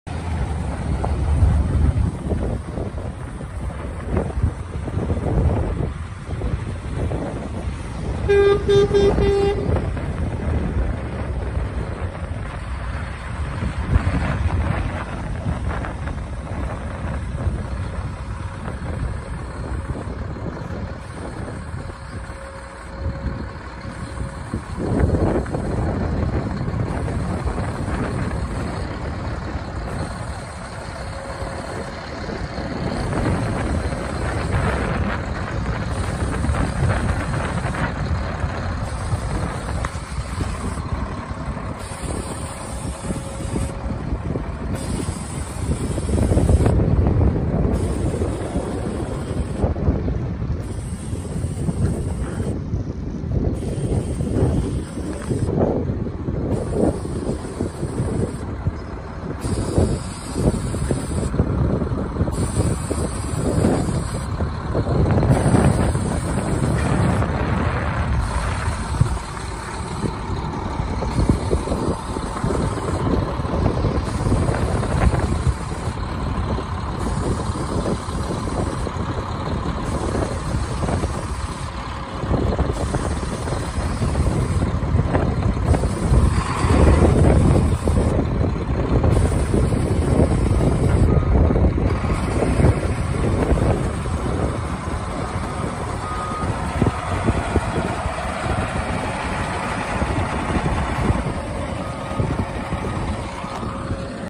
2016 VOLVO A25G For Sale
2016 VOLVO A25G, 4,400 Hours,  23.5R 25 Tires, Unit is working every day-Hours may vary-